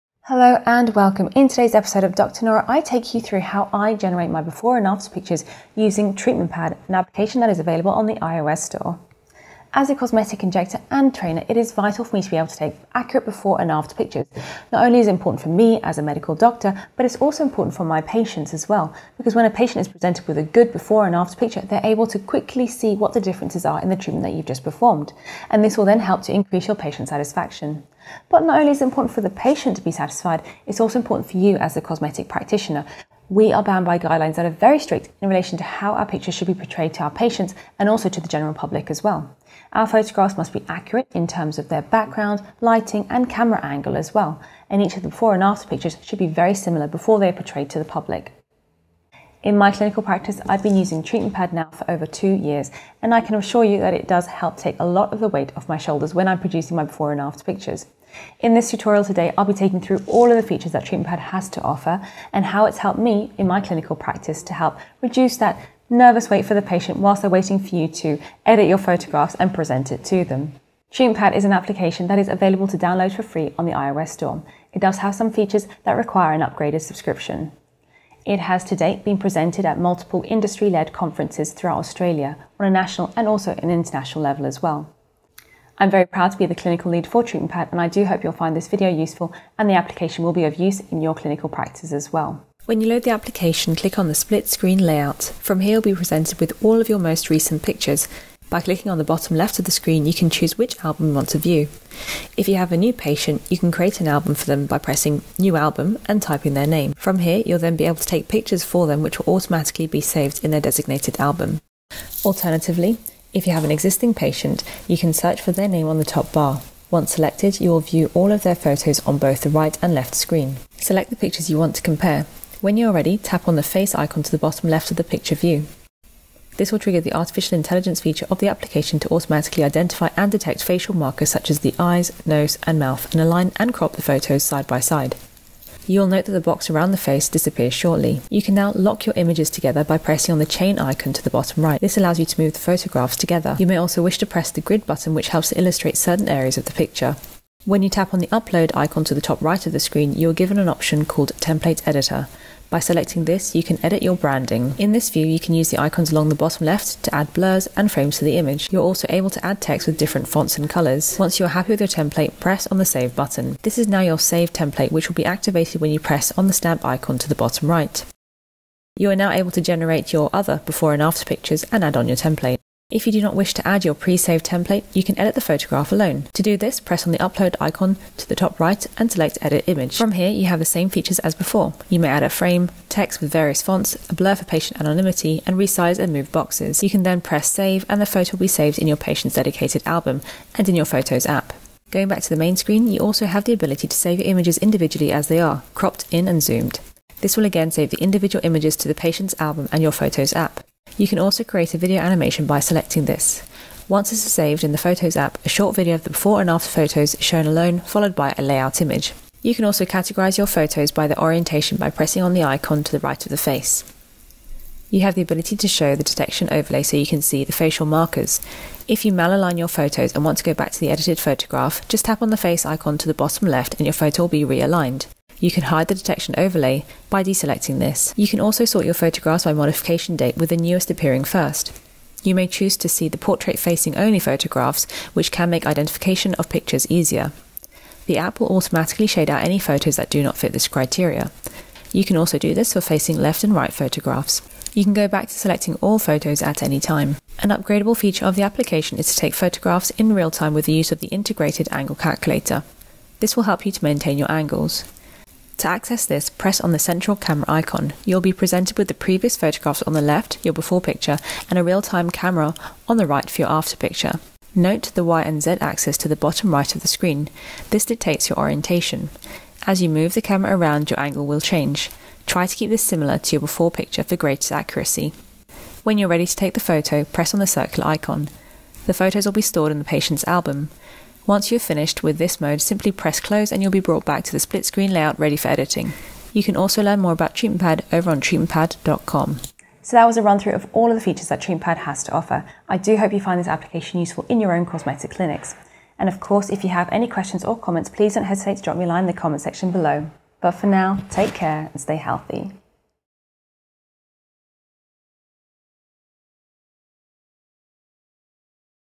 Treatment Pad Tutorial - How I Create Before and After Photos for My Patients
As a cosmetic injector, being able to show the patient results after treatment is not only important for assessing the changes, but it is also important in gaining a patient's trust. By being able to quickly and explicitly show the patient the benefits of their treatment, confusion is reduced and patient understanding is increased.

TREATMENT PAD
Treatment Pad is available to download through the iOS store and some features require an upgraded subscription. Treatment Pad uses Artificial Intelligence to automatically identify facial markers, such as the eyes, nose and mouth to align photographs at the tap of a button. It will also zoom in and crop photographs accordingly. Some advanced features of the app allow you to take accurate photographs in real time. On the split view screen you will be presented with the static before picture on the left and a live view on the right making it easy for comparison. In addition, the integrated angle calculator will identify the angle of the face to ease the accuracy.

Templates with branding can also be made through the application, with the ability to add frames, blurs and texts to the photographs ready for patient presentation or uploading to social media.

With limited training required to use the app and the ability to use on mobile devices Treatment Pad has had a lot of success globally. It has been presented in several industry events throughout Australia and boasts clinics using the application from New York, Los Angeles to Harley Street in London.

WHAT THE PRESS ARE SAYING?
With the ever-growing popularity of aesthetic treatments and facial sculpting injectables, producing quality before and after photos has never been more paramount for practitioners. Unfortunately, the slightest change in angle rotation or camera orientation can considerably change how the outcome of a procedure is portrayed in photos, thereby misrepresenting a practitioner’s good work.

LAYOUT EDITOR
- In the gallery view first select your before and after photos.
- Then press the facial alignment icon to use computer vision to mark and identify facial features detected in the photographs for automatic alignment.
- Use the optional grid overlay for micro-adjustments.
- Tap next to either save the collage to your device or share it.

CAMERA
- In the gallery view tap the camera icon at the bottom to launch the layout camera.
- Select photographs to compare with using the left pane.
- Take photographs with the camera using the right pane.
- Both panes have the facial alignment icon to aid alignment of the photographs.

TEXT EDITOR
- Tap on the upload button to select template editor
- Add blurs, frames and text
- Save the template to use later or generate a new template
- To access a saved template press on the stamp icon on the main view.